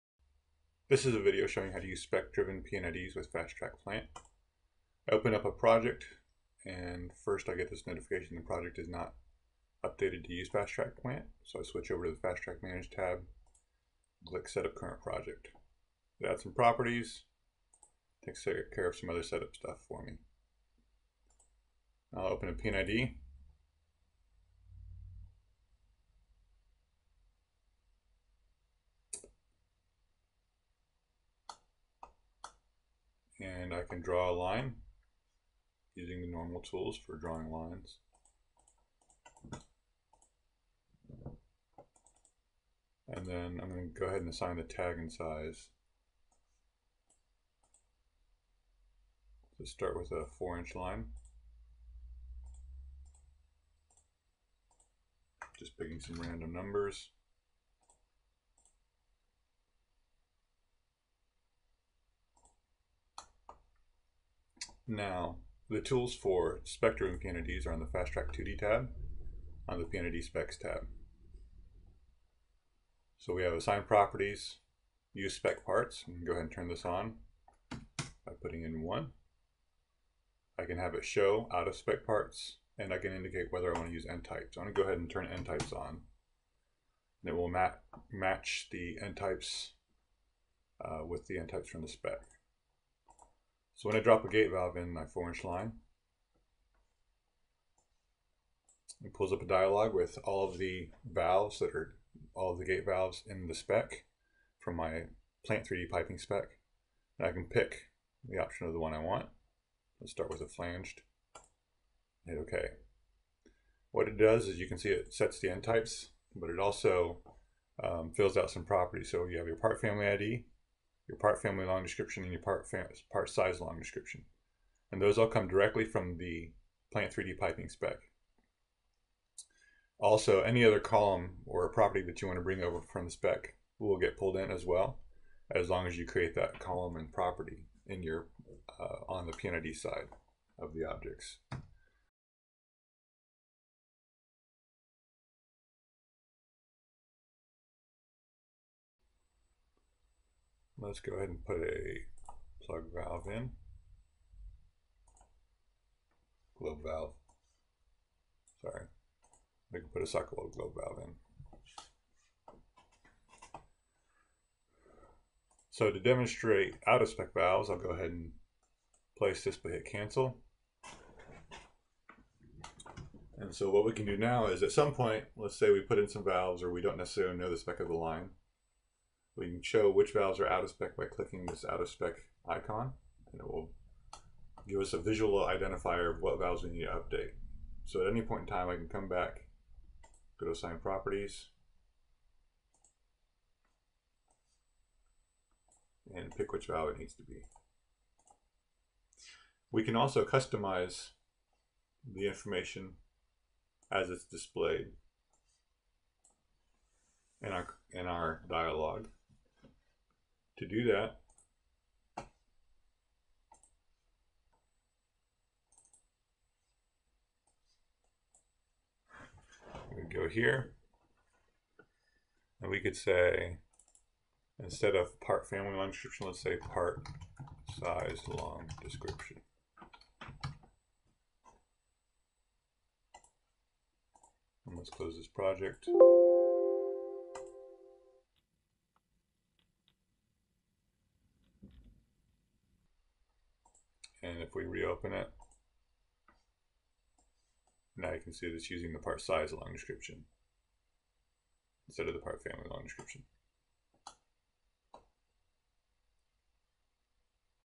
Spec Driven PnIDs in AutoCAD P&ID 2015-2017
See how to easily create spec driven P&IDs based on Plant 3d piping specs